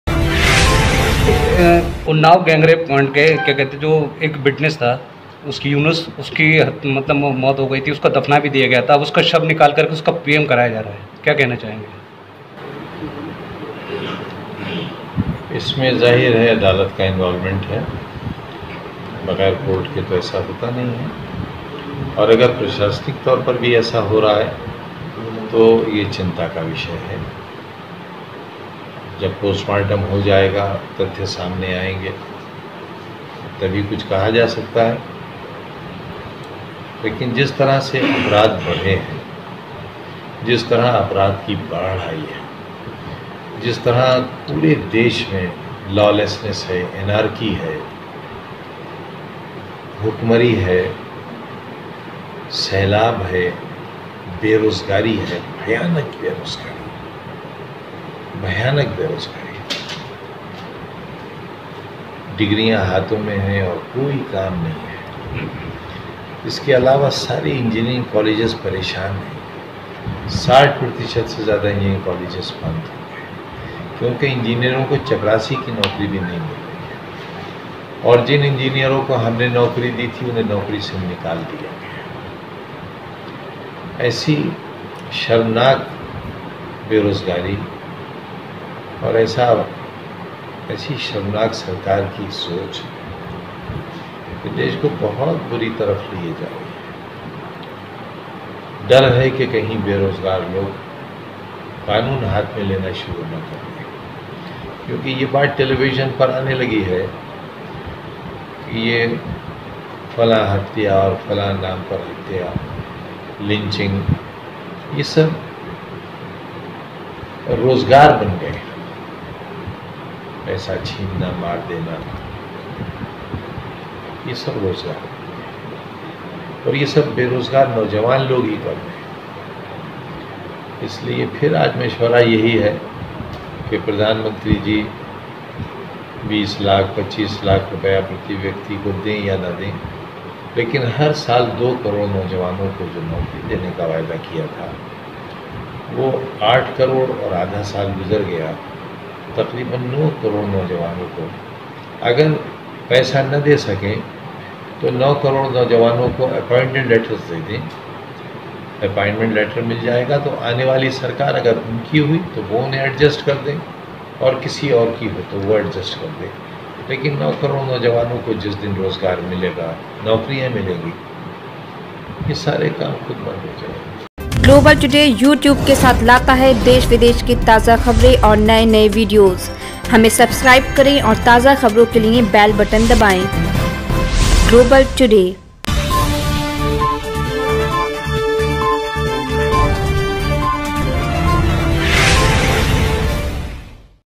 ,जिस तरह अपराध की बाढ़ आई है... तरह पूरे देश मे भुखमरी है
उन्नाव गैंग रेप कांड के विटनेस की मौत के बाद दफनाया गया अब उसका शव निकाल कर उसका पोस्टमार्टम कराया जा रहा है इस पर सपा नेता आज़म खान ने कहा इसमें अदालत का दखल है और बगैर कोर्ट के ऐसा होता नही है पोस्टमार्टम होने के बाद ही तथ्य सामने आयेंगे तभी कुछ कह जा सकता है लेकिन जिस तरह से अपराध बड़े है ,,,,जिस तरह अपराध की बाढ़ आई है ,,,,जिस तरह पूरे देश मे भुखमरी है ,,,सैलाब है ,,,बेरोज़गारी है ,,,डिग्रियां हाथो में है और कोई काम नही है इसके अलावा सारे इंजीनिरिंग कॉलेज परेशान है 60% से ज़्यादा इंजीनिरिंग कॉलेज बन्द हो चुके है किव के इंजीनियरों को चपरासी की नोकरी भी नही मिल रही है